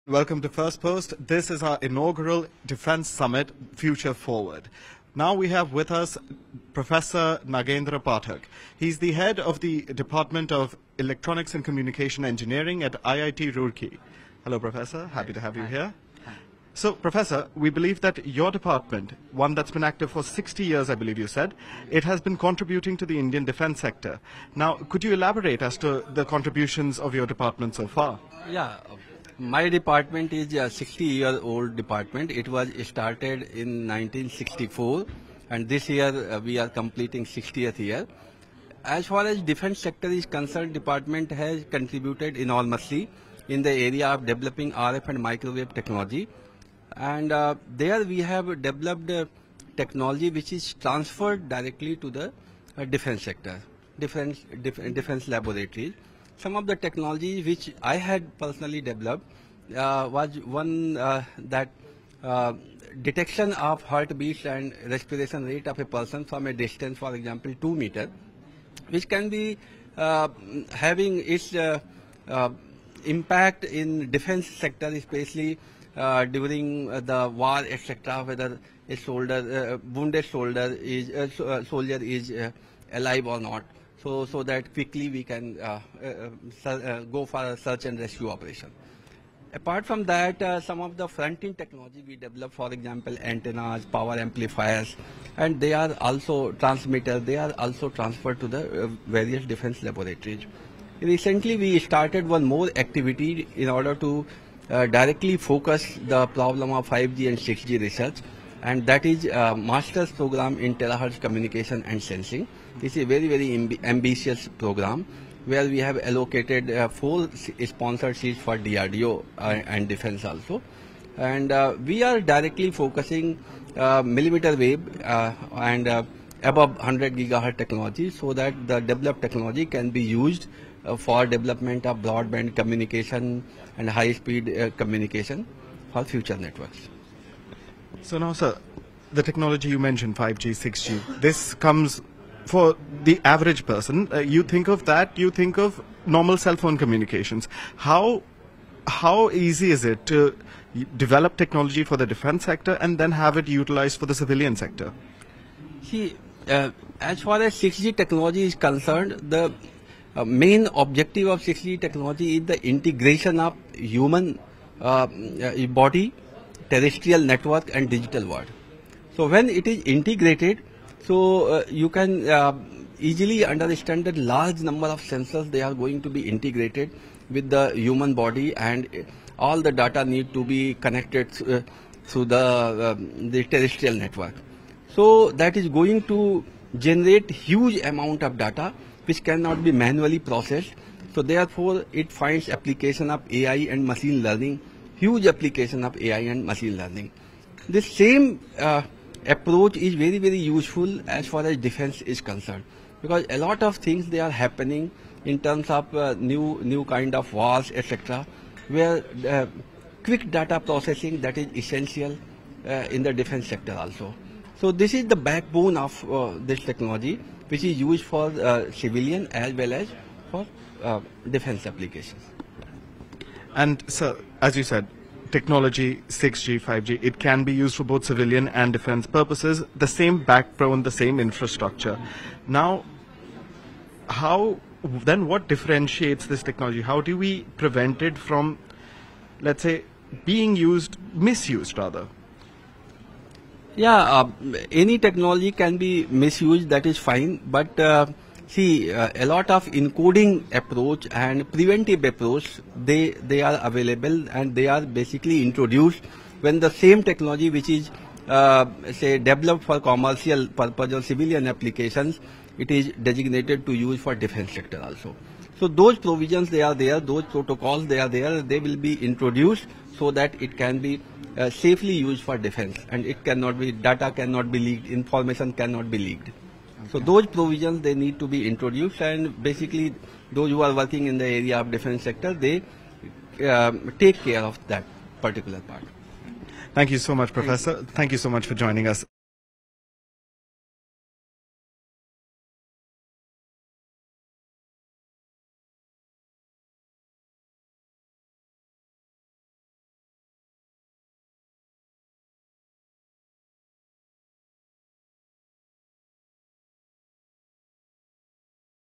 Should Artifical Intelligence Be Allowed in a War Zone? | Firstpost Defence Summit
IIT Roorkee Professor, Nagendra Prasad Pathak speaks about the application of Artificial Intelligence in the Defence Sector at the Firstpost Defence Summit 2024. While talking about the future of AI technology in defence, Professor Pathak explains why quick data processing is essential for the defence sector.